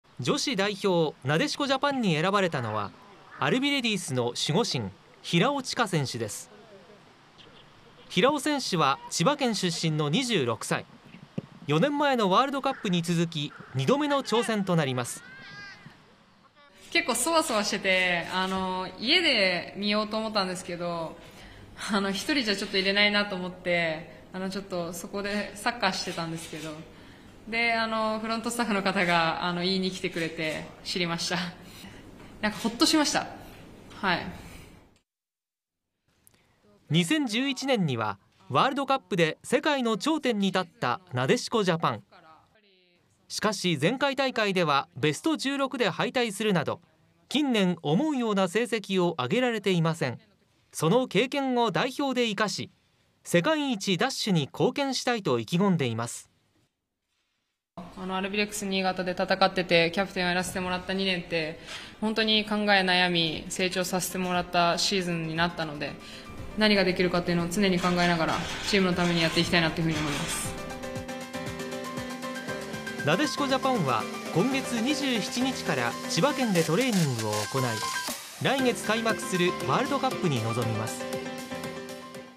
【サッカー女子】Ｗ杯メンバー発表 新潟の守護神・平尾知佳選手が代表入り “世界一奪取へ”意気込み アルビレックス新潟レディース
2023年6月13日放送

７月開幕するサッカー女子ワールドカップの代表メンバーが発表されました。アルビレックス新潟レディースのゴールキーパー平尾知佳選手が選ばれ、「大舞台」への意気込みを語りました。